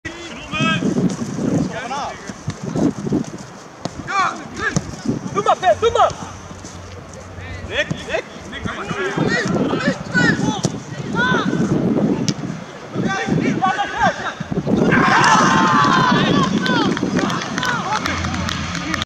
2-0 Nesjet kopt voorzet speler Walburgia binnen
SC Leeuwen - Walburgia